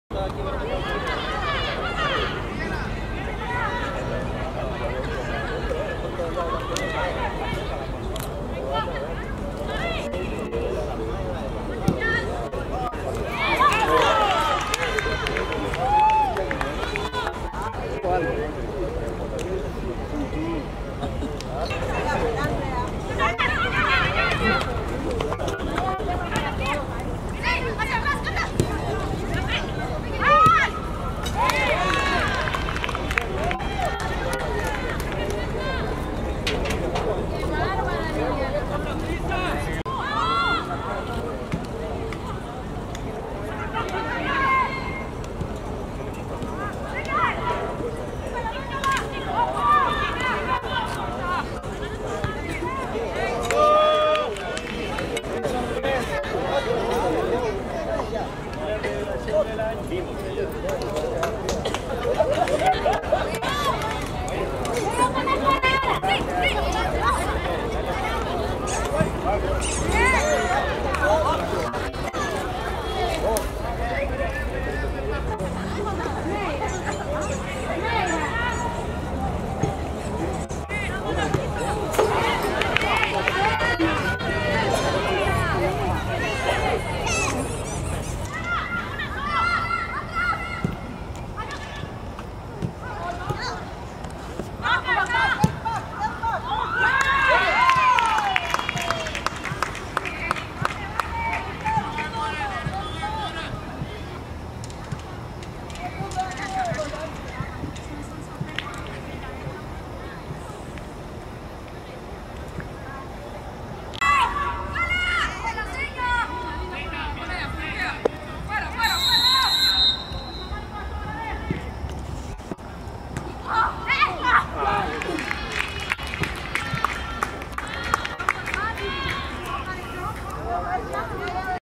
Saprissa FF avanza a las semifinales de Copa Uncaf
Saprissa FF se clasificó a las semifinales de la Copa Interclubes de la Uncaf, en Managua, Nicaragua. Así lo aseguró al golear 7-0 al Jewel Fury FC (Belice), con tantos de María José Pereira, Carolina Venegas (2), María José Brenes (2), Sofía Varela y María Paula Salas.